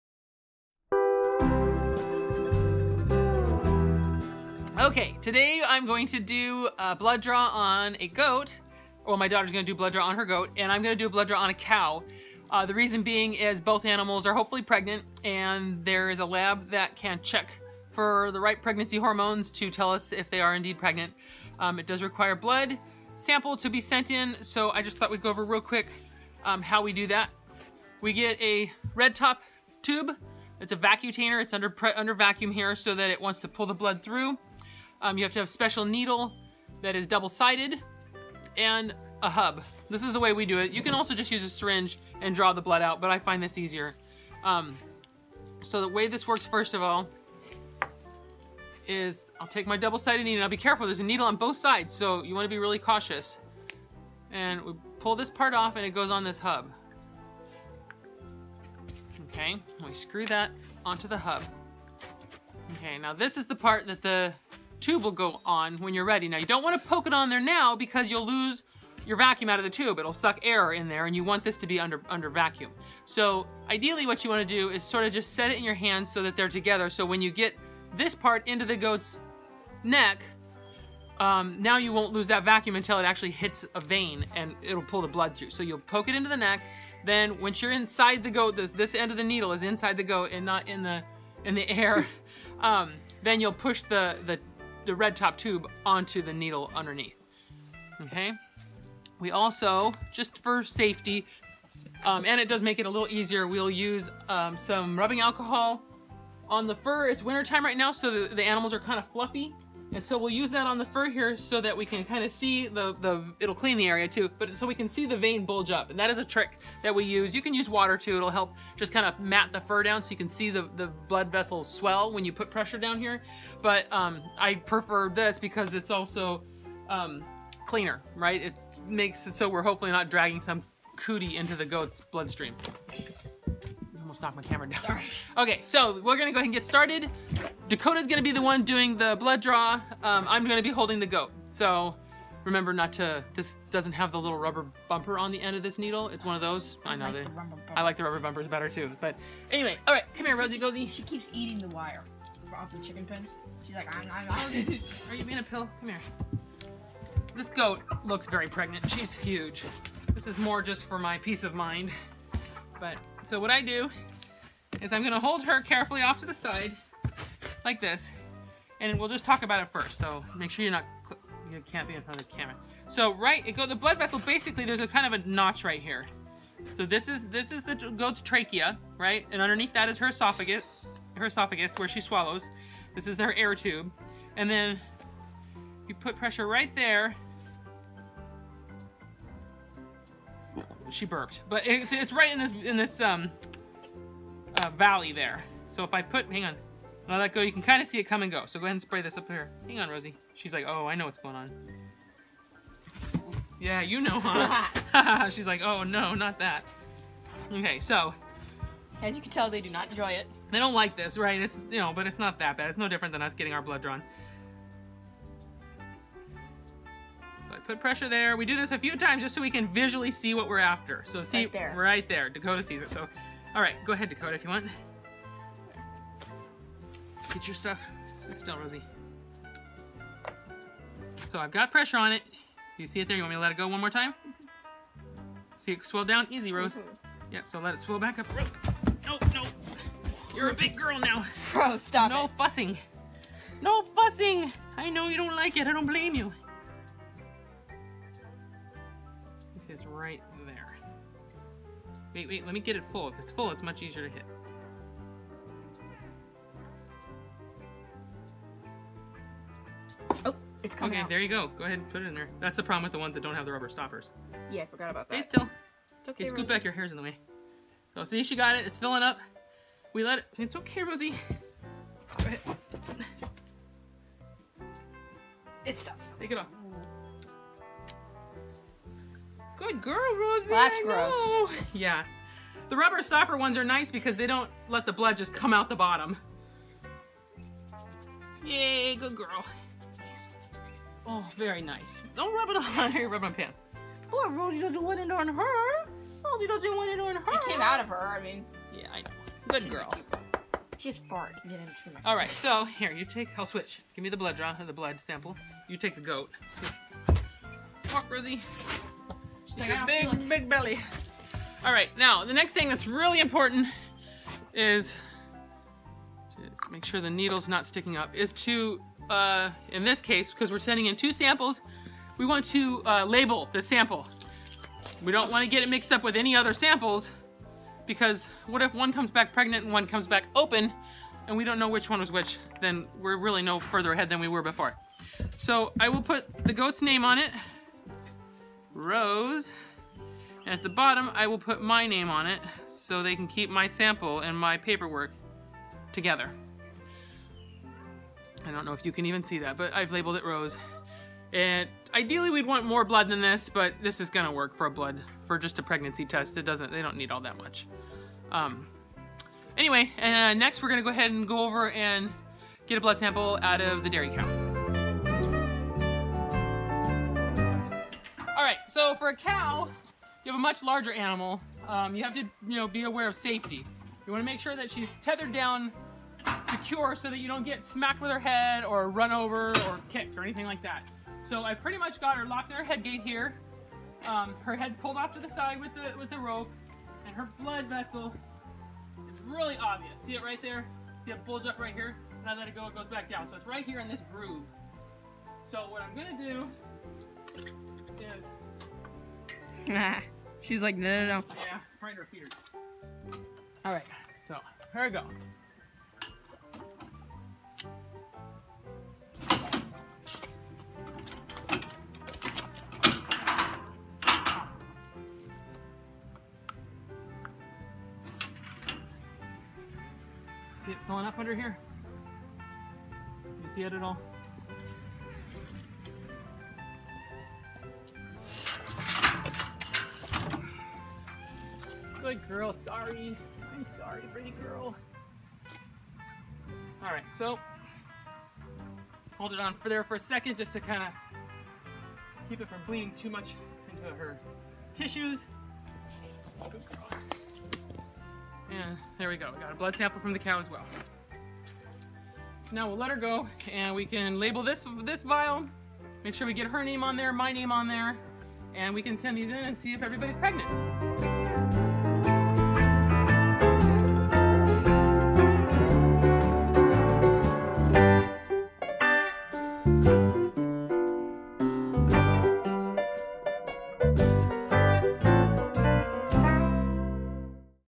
Drawing Blood Sample from Goat and Cow
In this video, my 12 year old daughter draws blood from her goat and I draw blood from my dairy cow so that we can send the samples in to be pregnancy tested.  There's no need to call a vet out to get a blood sample for most lab work when it's a simple task to accomplish on your own!  You can pick up the needed supplies from your vet for next to nothing, and submitting to a lab is as simple as filling out the paperwork and popping the samples into a USPS Priority box!